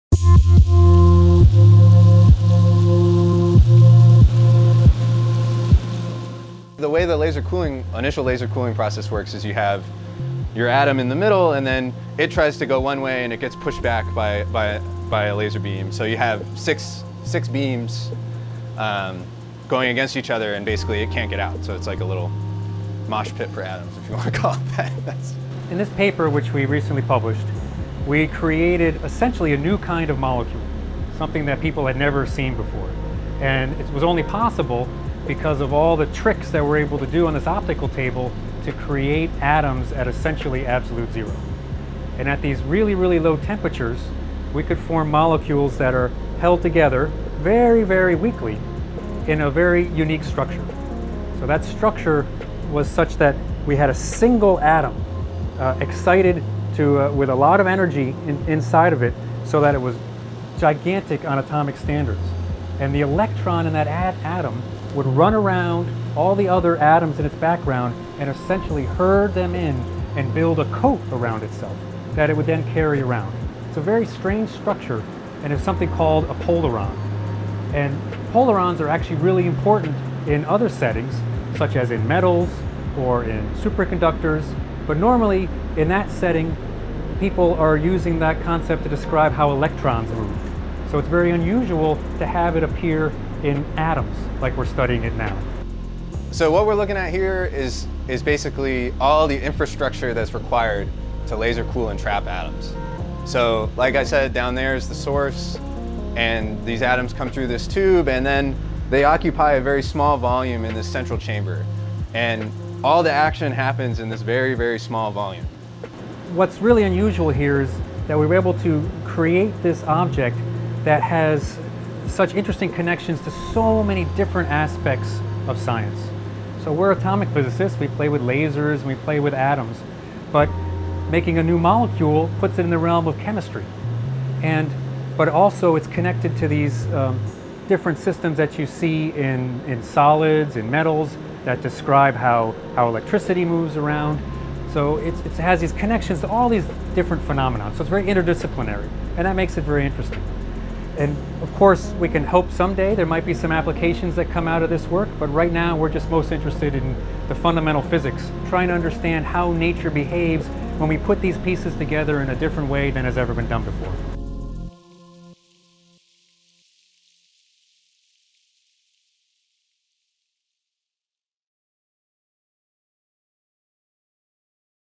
Physicists build bizarre molecules called 'Rydberg polarons'
Using lasers, U.S. and Austrian physicists have coaxed ultracold strontium atoms into complex structures unlike any previously seen in nature. "I am amazed that we've discovered a new way that atoms assemble," said Rice University physicist Tom Killian. "It shows how rich the laws of physics and chemistry can be." Killian is the lead scientist on a new paper in Physical Review Letters (PRL) that summarized the group's experimental findings.
Killian teamed with experimental physicists from Rice's Center for Quantum Materials and theoretical physicists from Harvard University and Vienna University of Technology on the two-year project to create "Rydberg polarons" out of strontium atoms that were at least 1 million times colder than deep space.